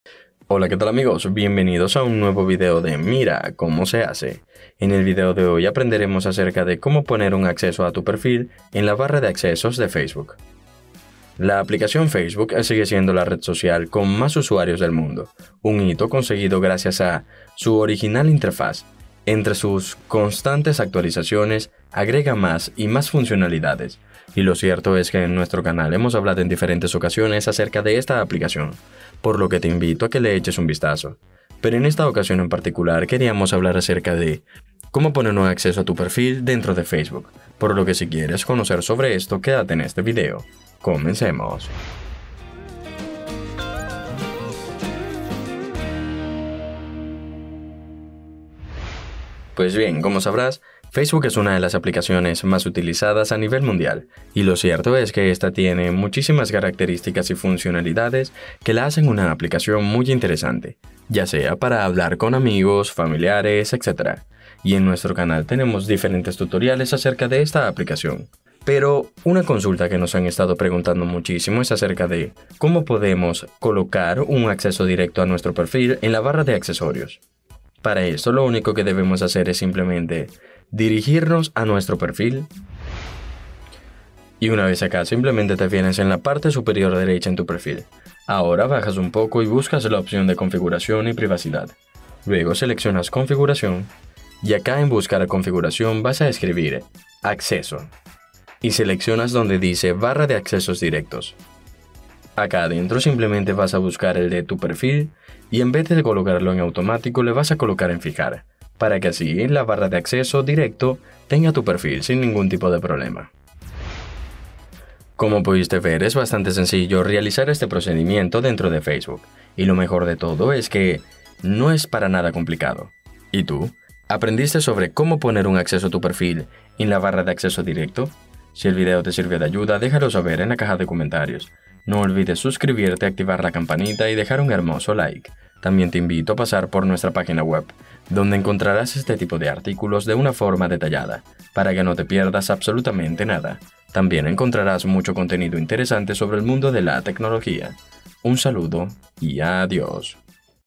¿Cómo poner un acceso a tu perfil en la 'Barra de accesos' de FACEBOOK?
En este video te mostramos ¿Cómo poner un acceso a tu perfil en la 'Barra de accesos' de FACEBOOK? En este canal encontrarás los mejores tutoriales. ¡No te lo pierdas!